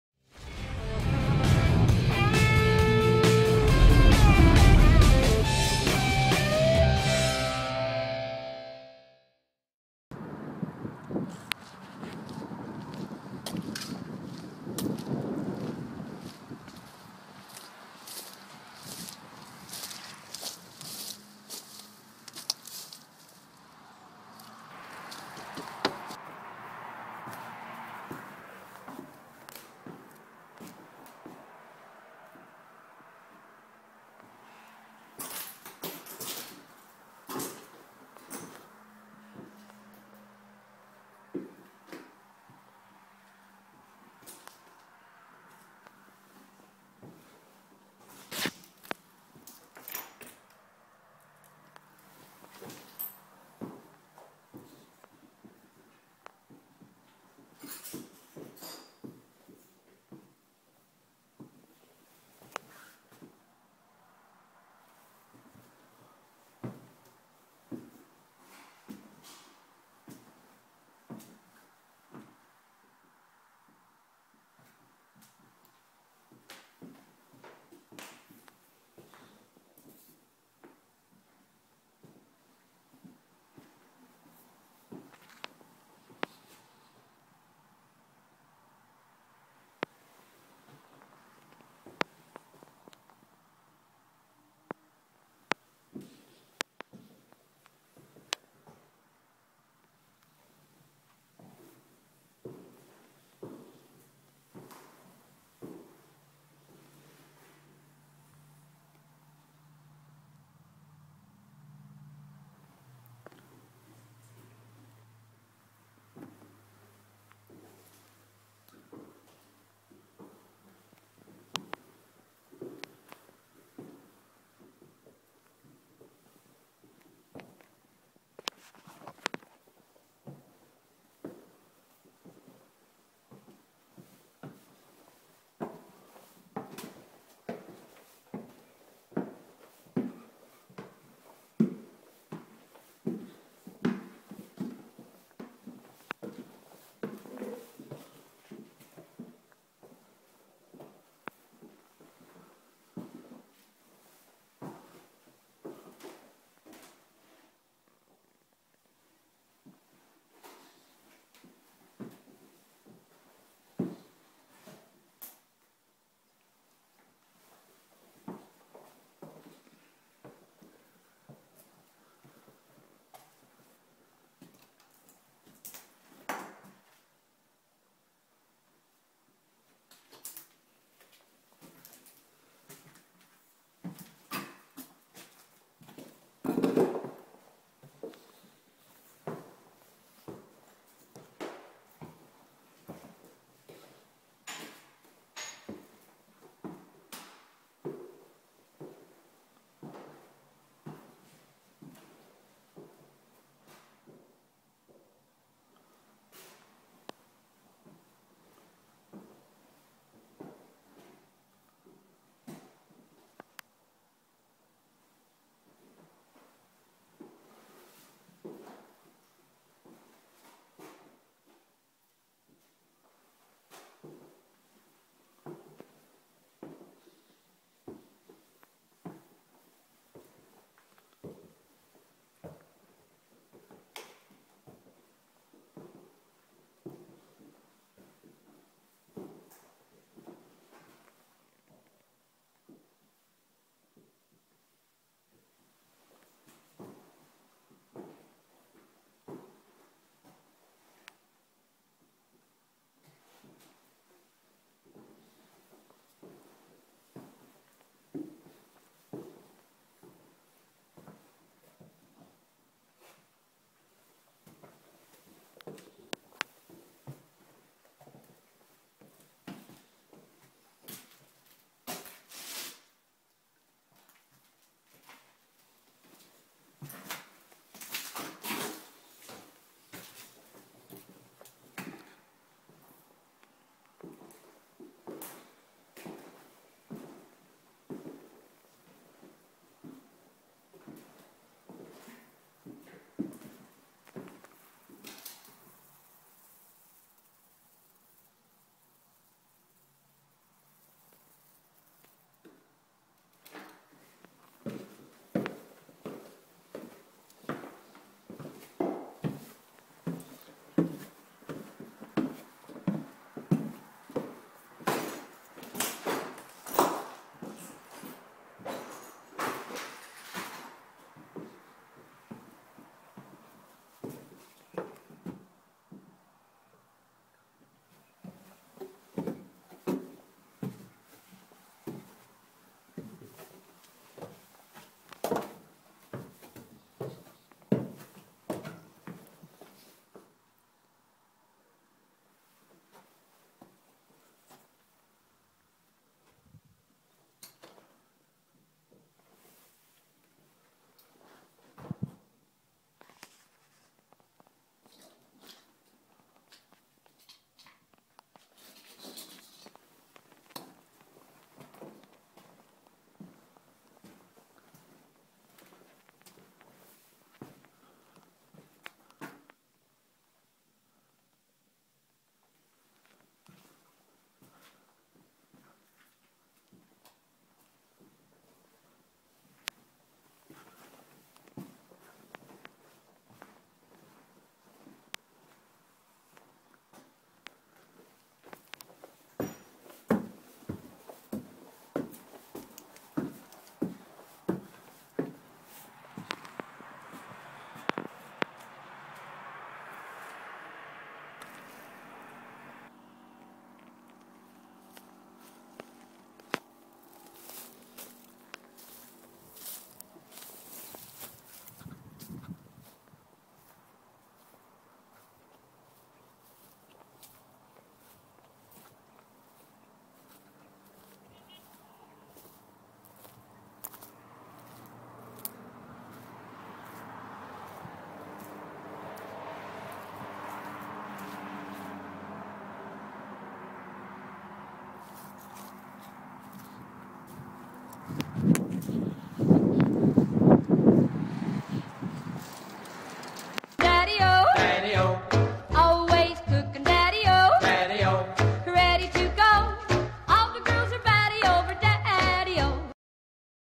Exploring an Abandoned 19th Century Farm House at Taunton Rd. and Lakeridge in Whitby, Ontario
WingmanJan and I waited to gain access to this one for quite some time.  We had visited this property on 2 previous occasions then finally, an open door!

This late 19th Century farm house was fantastic and  cared for exactly how a house of this beauty should be.  Lots of renovations without ruining that farm house feel. Dual staircases (which are my favorite), and classic hand crafted banisters and baseboards adorn this place. Sitting on a huge piece of land this place was in the way of the Hwy 407 ETR construction and was demolished shortly after this video was filmed.

This video is from March 2013 and I have only now got around to editing it.  So the video will be more shaky cause I was using the Ipad to film and not my GoPro.  Also my batteries were fading on my flashlight so the footage in the basement is not as clear as I would have liked

Thank you so much for taking an interest in my videos!